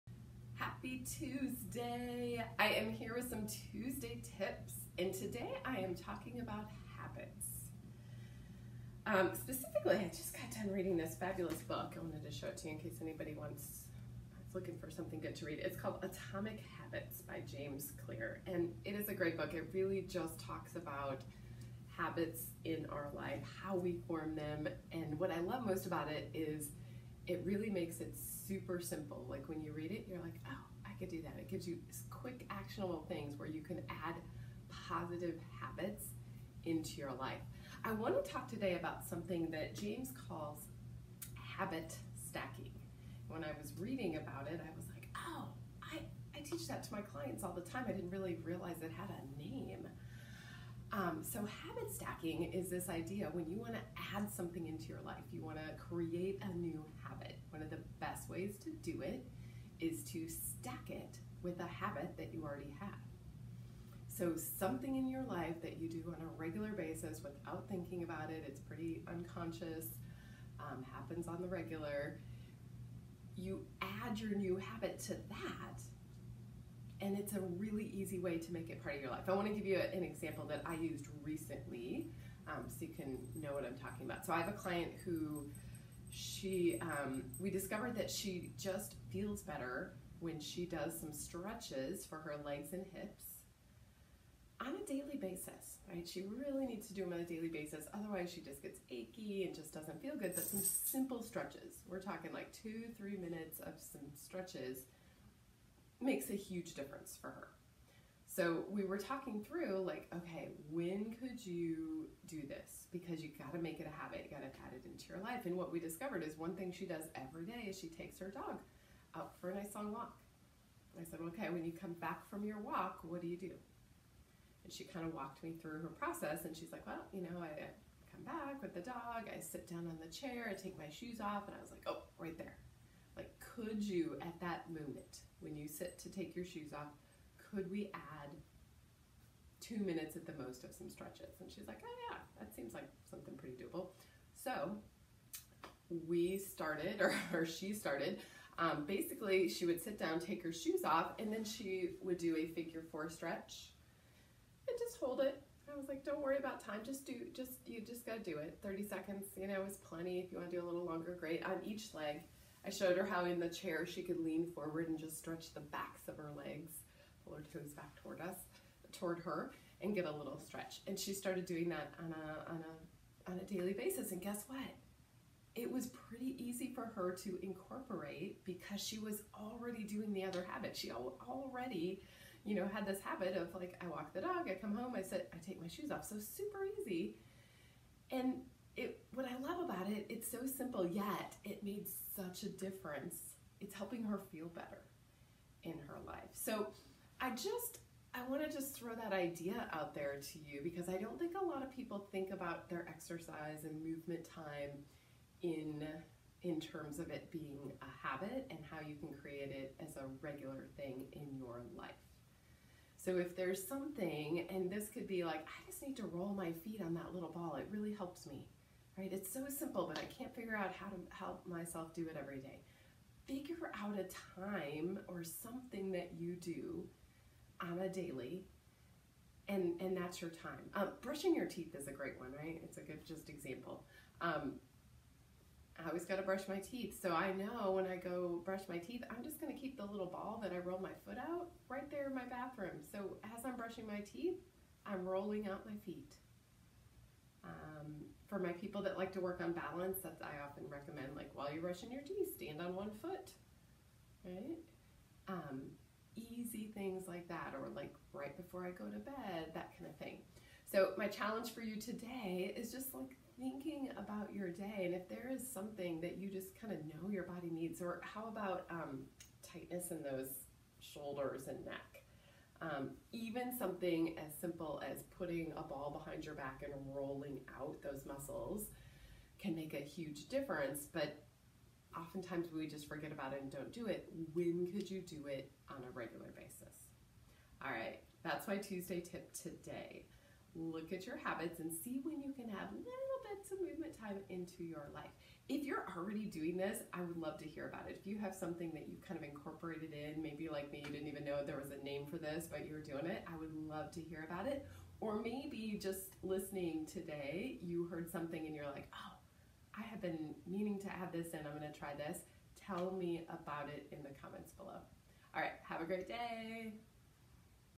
Tuesday Tips: Learning to do this simple thing can keep your body feeling fabulous!
The things we need to do to feel good in our body are not usually hard...but they do require we actually do them on a regular basis to be effective. Learn how you can easily incorporate healthy movement into your life with today’s simple tip!